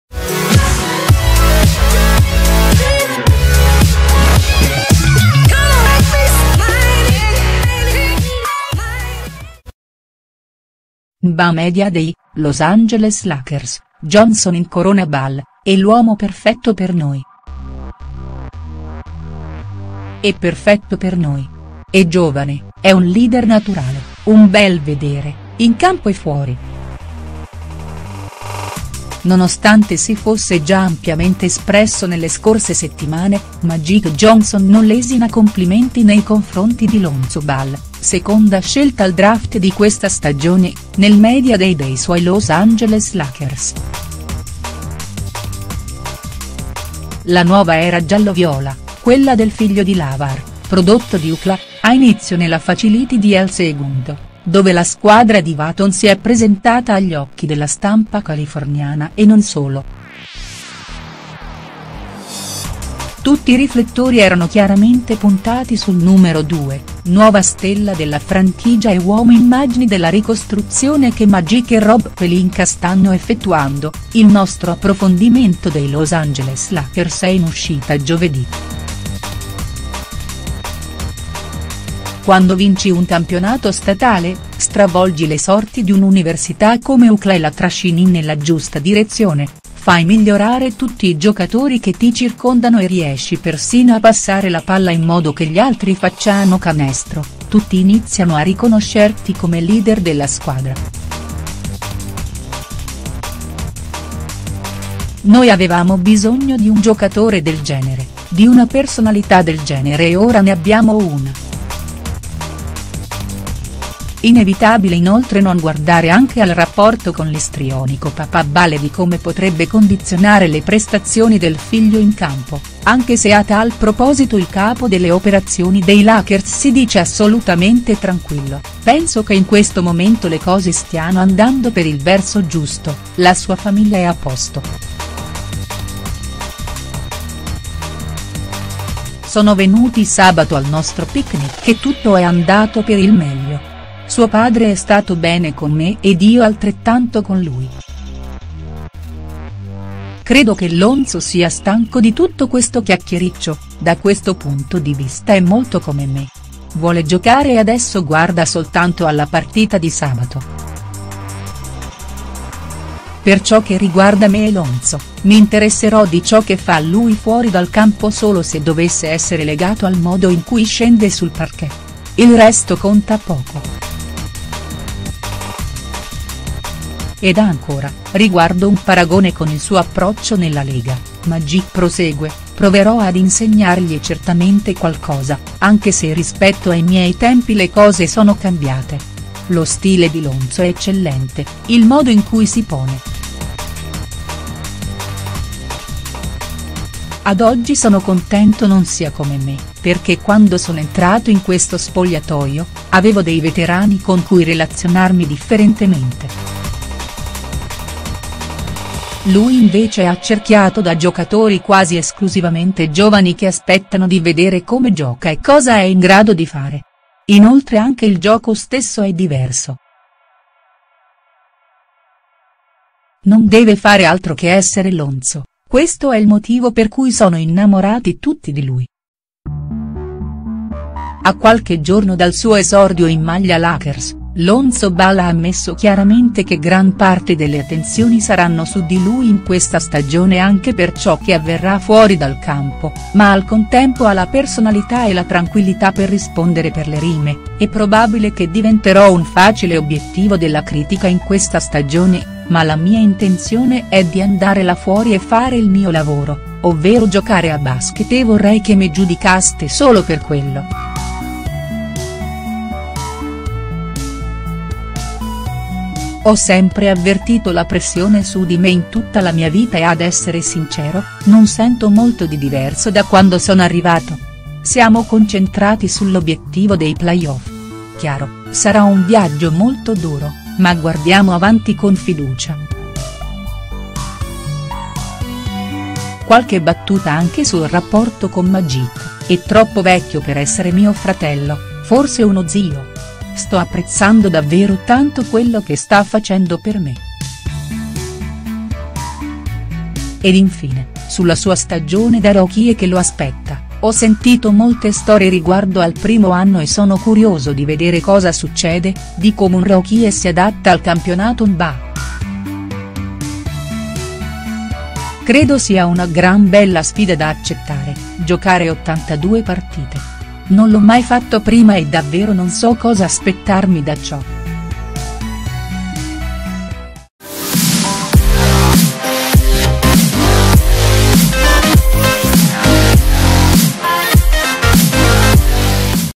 NBA Media Day - Los Angeles Lakers, Johnson incorona Ball: "E' l'uomo perfetto per noi".HD
Il presidente delle operazioni dei Los Angeles Lakers, nel giorno di presentazione della squadra ai media statunitensi e non solo, ha parlato della nuova stagione e soprattutto del nuovo gioiello di casa gialloviola, Lonzo Ball.